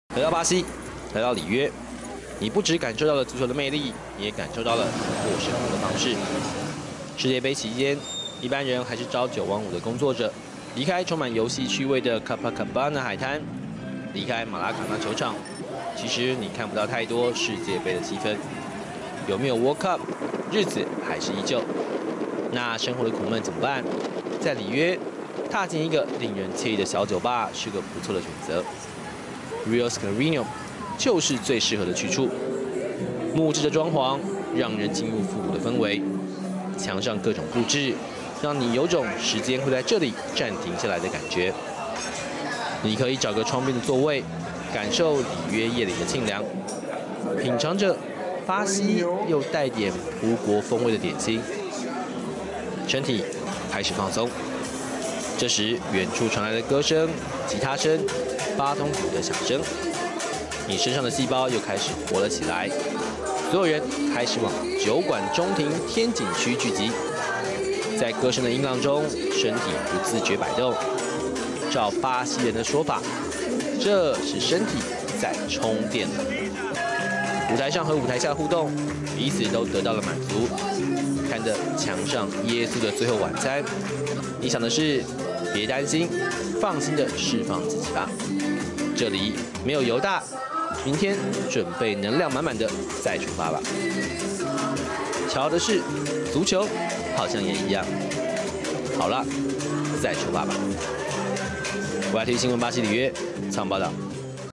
7/3 足球不是全部生活 世足賽外還有人生
世界盃的八強名單已經全部出列了
,你支持的球隊是否繼續挺進還是已經下台一鞠躬,接下來休兵的幾天,巴西人的生活也會暫時恢復常態,但是那勝負後的感傷,以及生活中的壓力 也會隨之重新浮現,那該怎麼辦呢?巴西人有他們的充電之道.